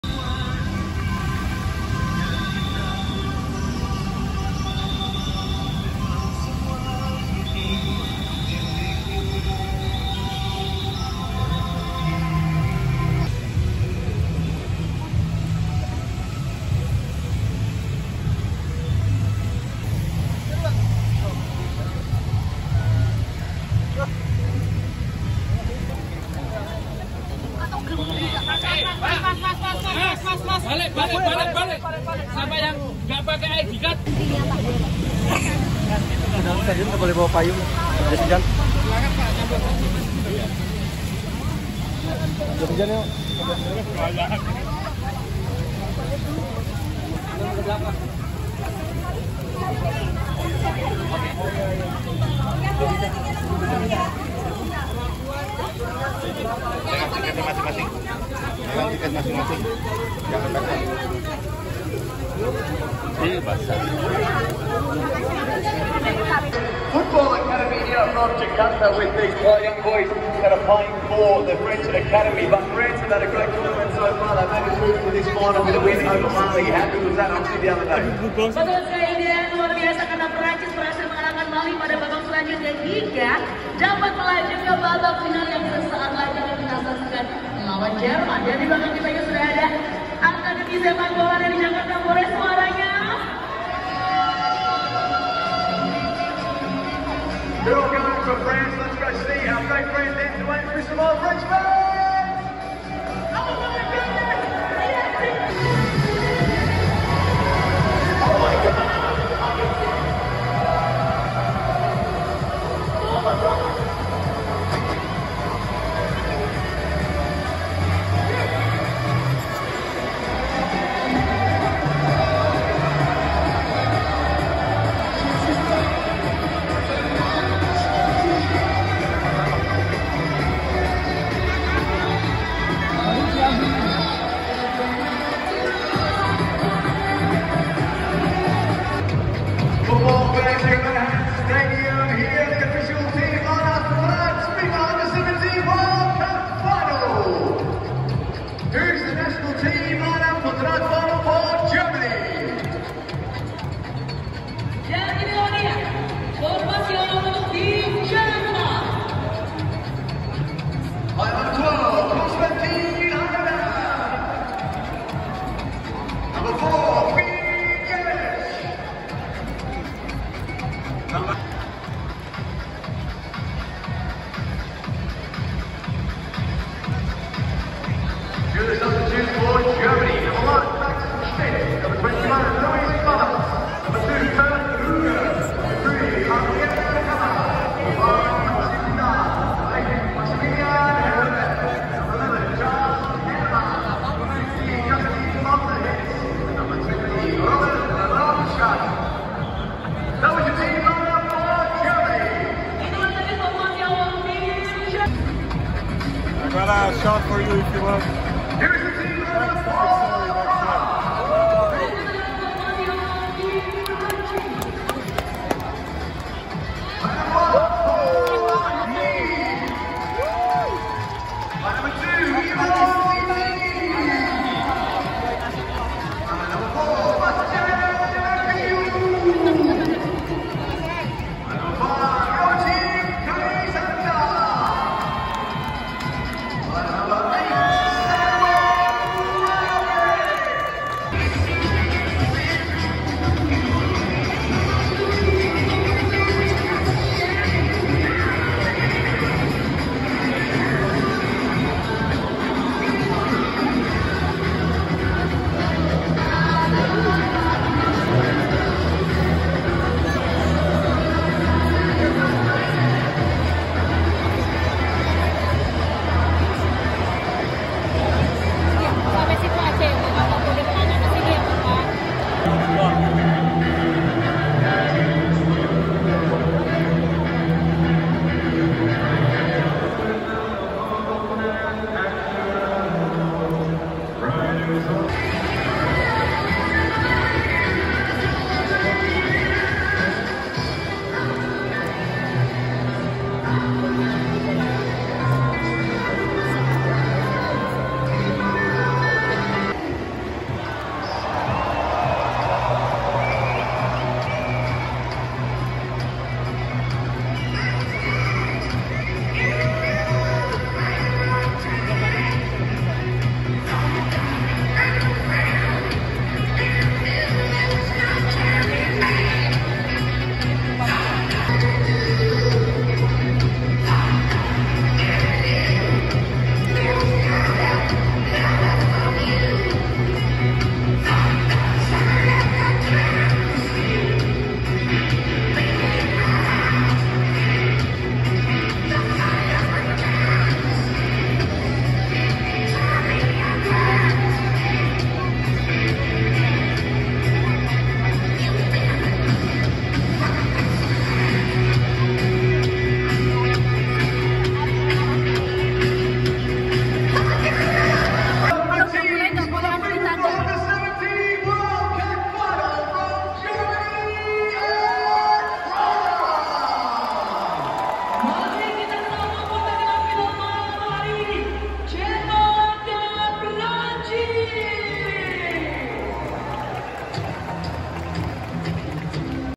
SUASANA FINAL PIALA DUNIA U-17 DI STADION MANAHAN SOLO ..‼️ Sejarah Baru Tercipta ..🔥🔥
Suasana Menjelang Final Piala Dunia U-17 di Stadion Manahan Solo.
Jerman U-17 vs Prancis U-17

Stadion Manahan Solo
Kick Off 19.00 Wib

Bravo Sepakbola